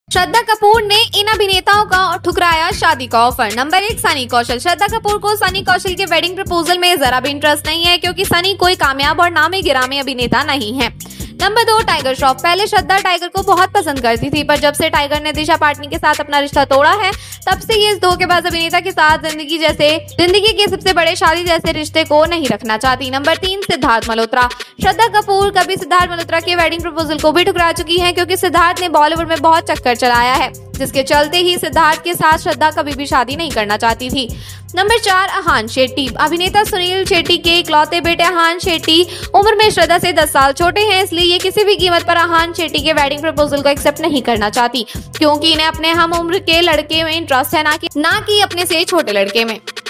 श्रद्धा कपूर ने इन अभिनेताओं का ठुकराया प्रपोजल....
Shraddha Kapoor rejected the proposal of these actors.